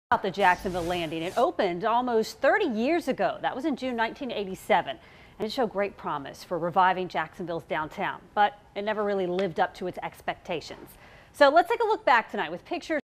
Moving Newsroom Shot WTLV/WJXX Jacksonville
Making our newsroom shot more exciting by adding a pan and Video Over the Shoulder